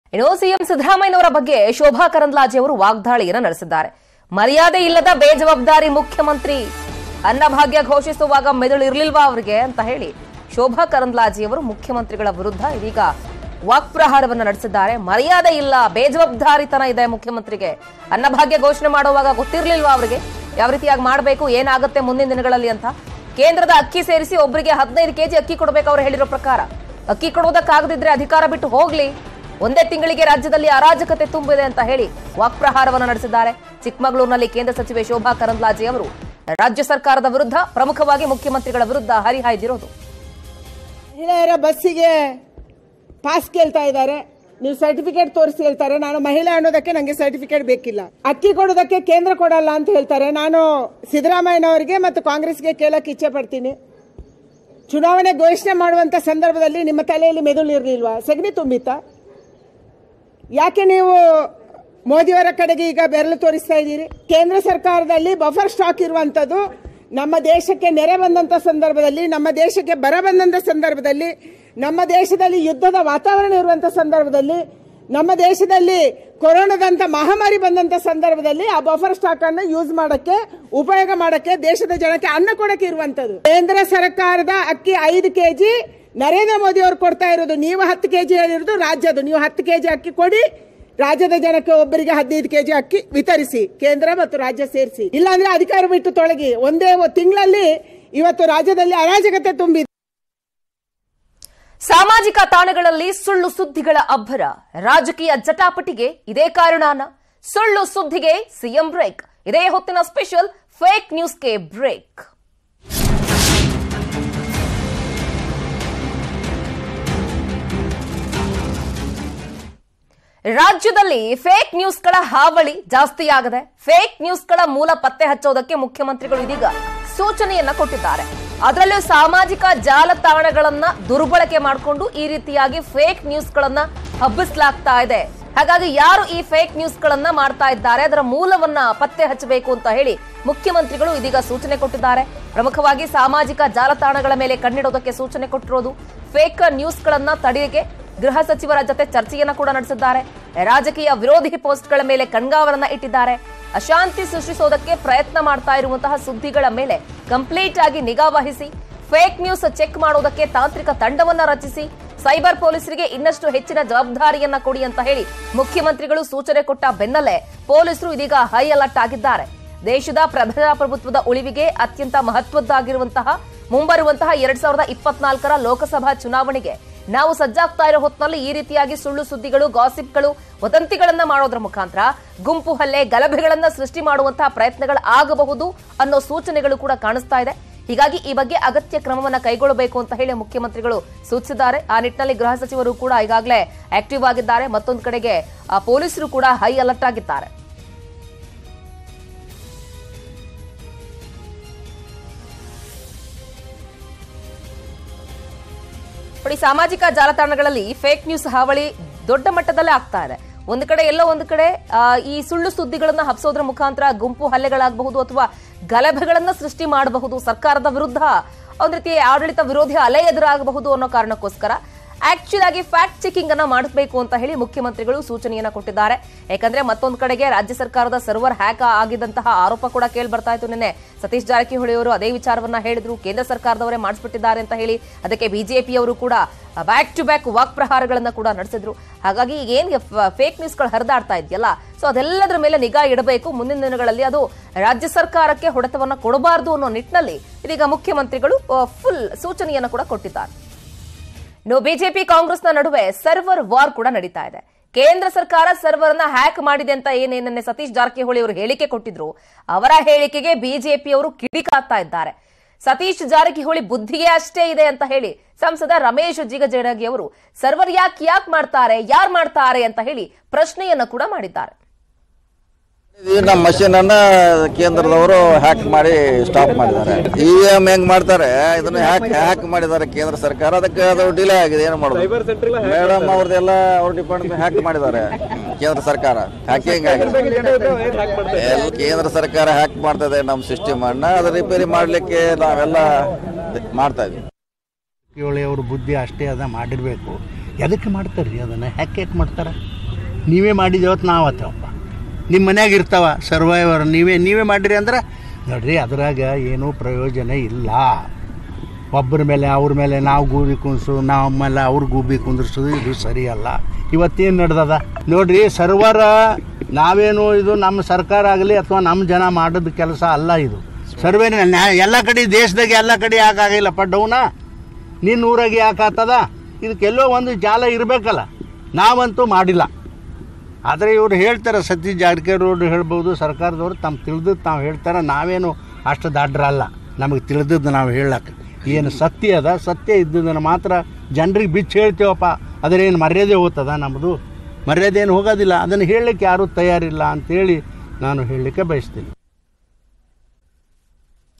Shobha Karandlaje Slams CM Siddaramaiah | ‘ಮರ್ಯಾದೆ ಇಲ್ಲದ ಬೇಜವಾಬ್ದಾರಿ ಮುಖ್ಯಮಂತ್ರಿ’
Shobha Karandlaje Slams CM Siddaramaiah | ಕೇಂದ್ರದ ಅಕ್ಕಿ ಸೇರಿಸಿ ಒಬ್ಬರಿಗೆ 15 ಕೆ.ಜಿ.ಅಕ್ಕಿ ಕೊಡಬೇಕು.
ಅಕ್ಕಿ ಕೊಡಲು ಆಗದಿದ್ದರೆ ಅಧಿಕಾರ ಬಿಟ್ಟು ತೊಲಗಿ. ಒಂದೇ ತಿಂಗಳಿಗೆ ರಾಜ್ಯದಲ್ಲಿ ಅರಾಜಕತೆ ತುಂಬಿದೆ.  ಸಿಎಂ ಸಿದ್ದರಾಮಯ್ಯ ಬಗ್ಗೆ ಶೋಭಾ ಕರಂದ್ಲಾಜೆ ವಾಗ್ದಾಳಿ